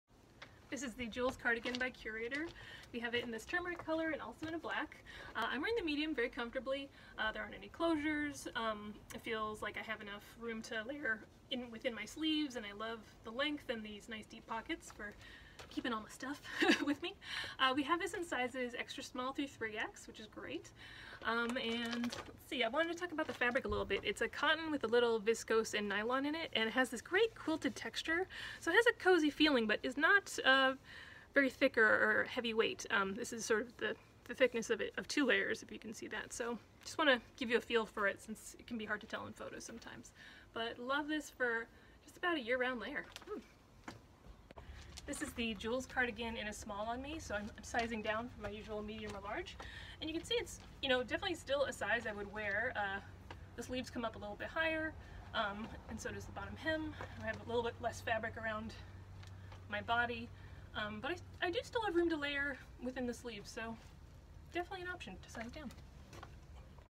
Velouria Fit Talks - Jules Cardigan in Turmeric
Chika tells you about the fit and feel of the Jules Cardigan and compares the M (her usual size) and the S (one size down). Also available in black.